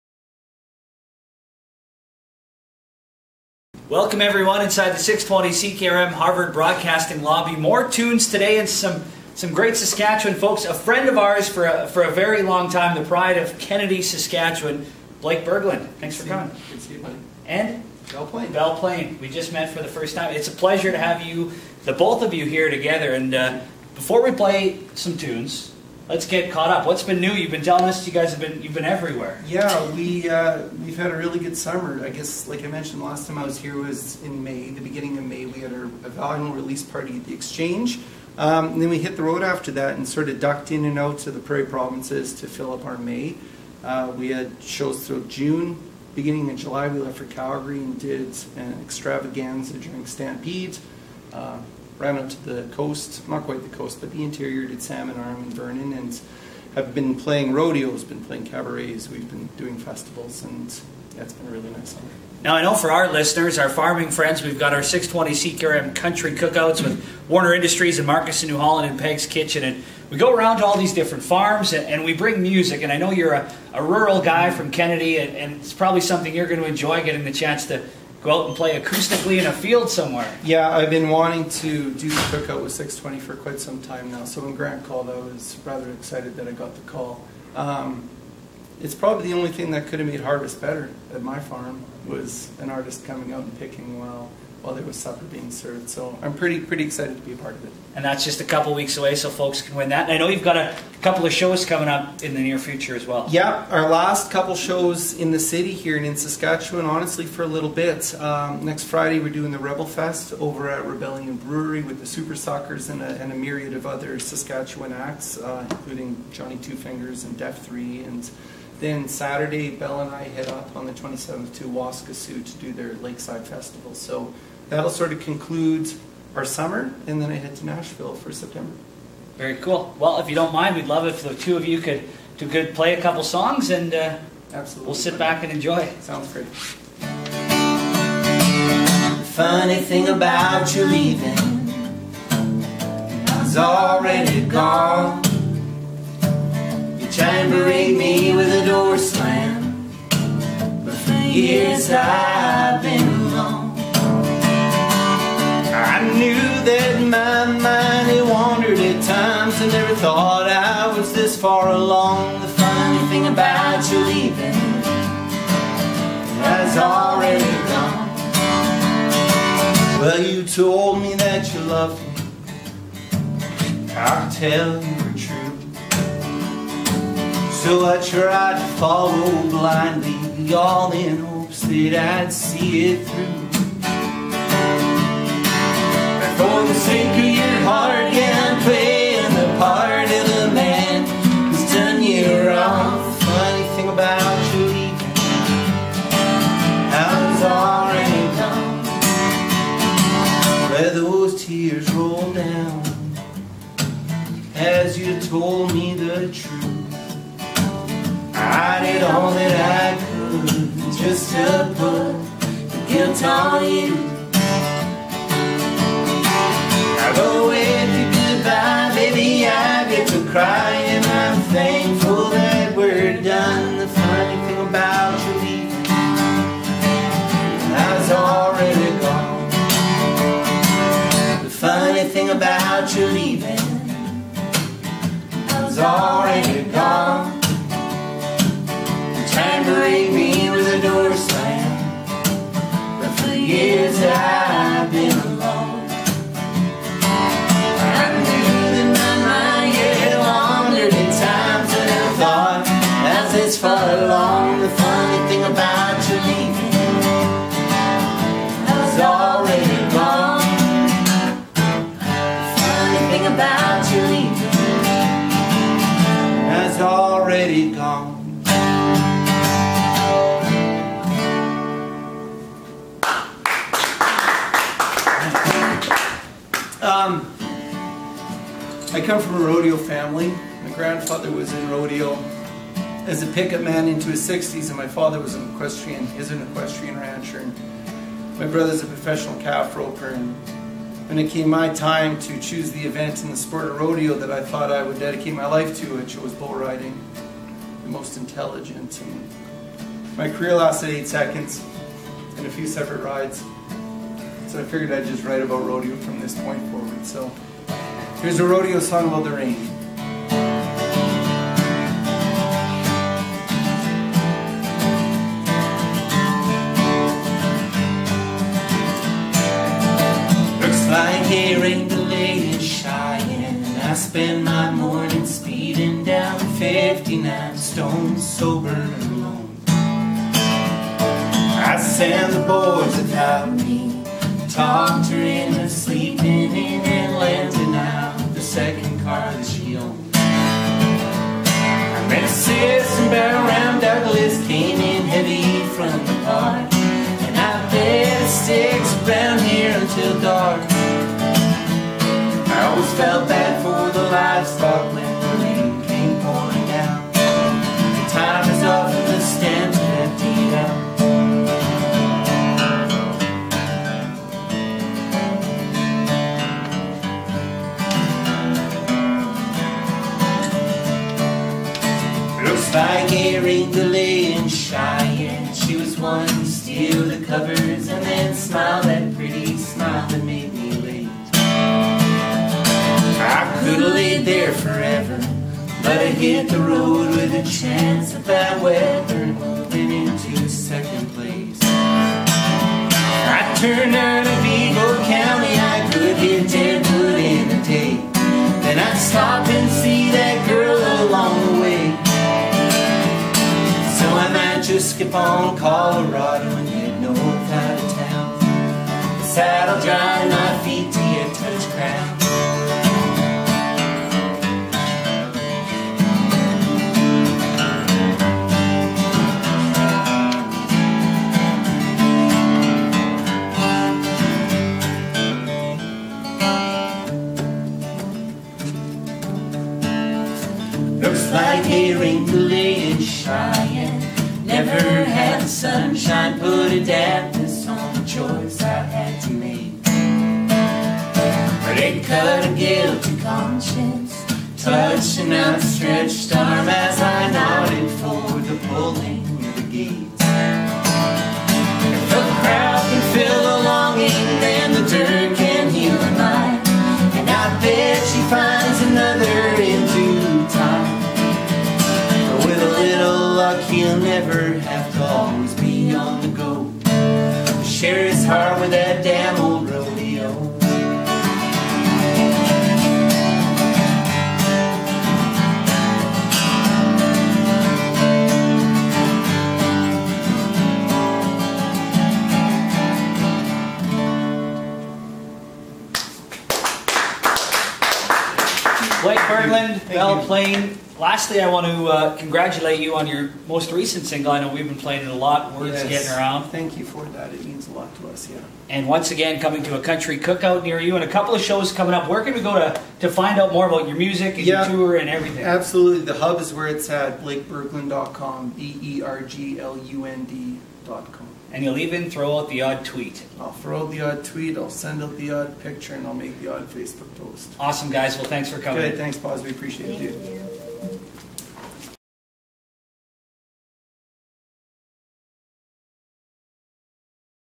CKRM Lobby Jam - Blake Berglund
Just a few more dates to wind up a busy summer for Mr. BLAKE BERGLUND. He'll be at the Rebel Heart Music Fest, the Waskesiu Lakeside Music Festival, and before he takes off to Nashville on business, we're so thankful that he's freed up an evening to join us on this year's first CKRM Country Cookout! Blake swung by the lobby for a quick catch-up and graced us with a few tunes accompanied by the always lovely Belle Plaine. Check it out!